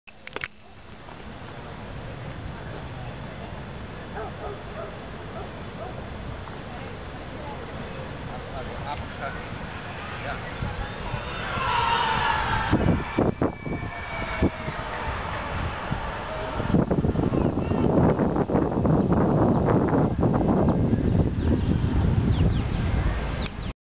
Huaca Huallamarca, 2
Vista del perímetro circundante de la Huaca Huallmarca, San Isidro, Lima, 23 de julio del 2011, 3.57 pm.